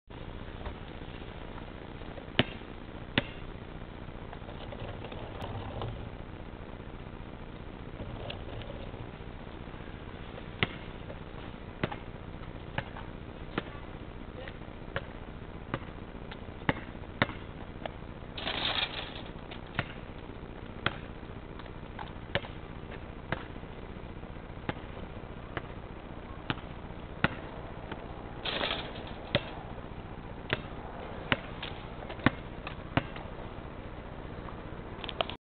Dunking over a trashcan
me dunking over a trashcan its about 3feet tall